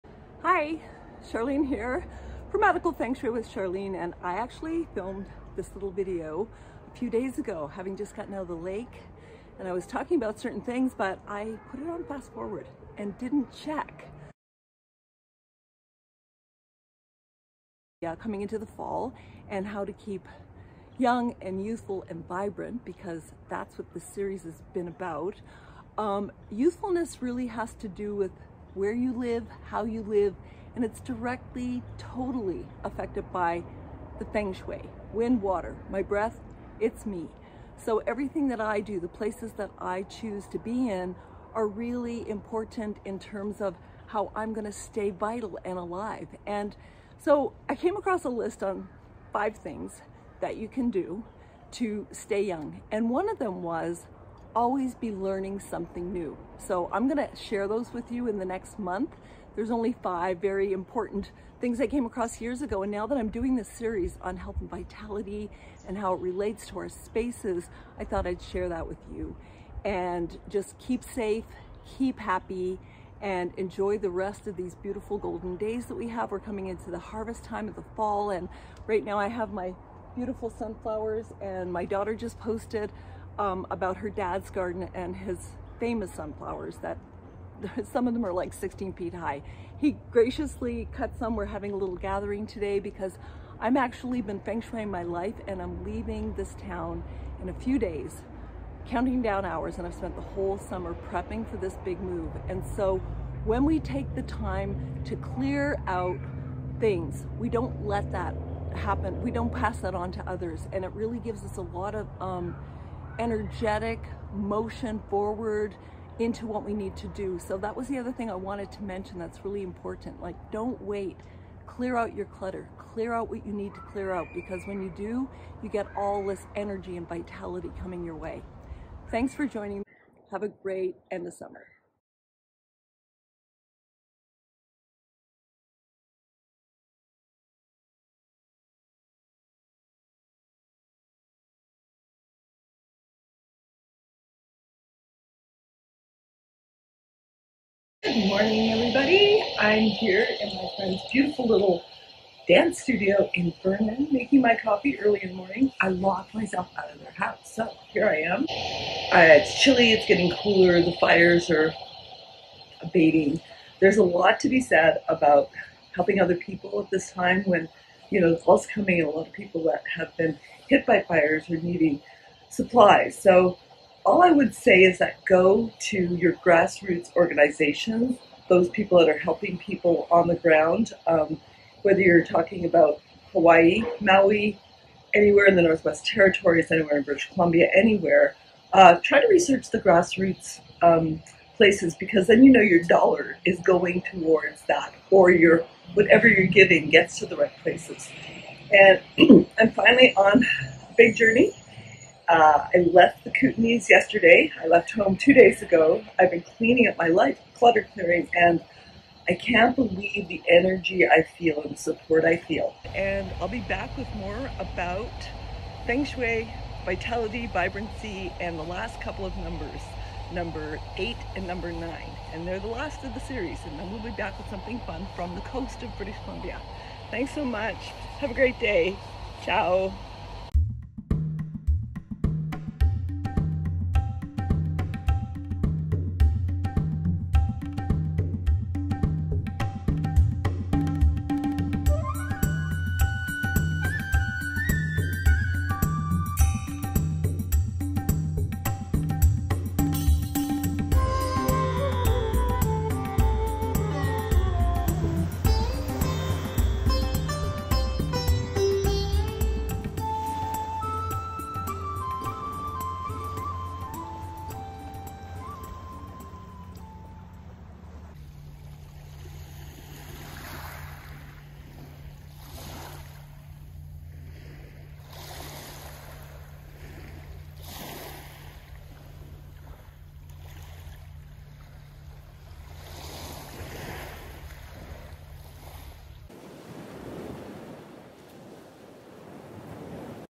Feng Shui Bliss ~ End of Summer Interlude ~ Shift, Reset & Retreat for Rejuvenation
Having Feng Shui-ed my home and shifted my life to make a major move, this  'End of Summer ~ Feng Shui Bliss' shares how letting go creates energy for new things. For me, a poignant break in the  Health, Vitality & Youthfulness Series, I encourage all who have empty nests etc, overpacked storage areas, etc  to size down and clutter clear NOW. This summer for me has been 'living my work' & 'walking my talk'. The process has energized and revitalized me for the next steps. More fun videos on my travels in October! Cheers.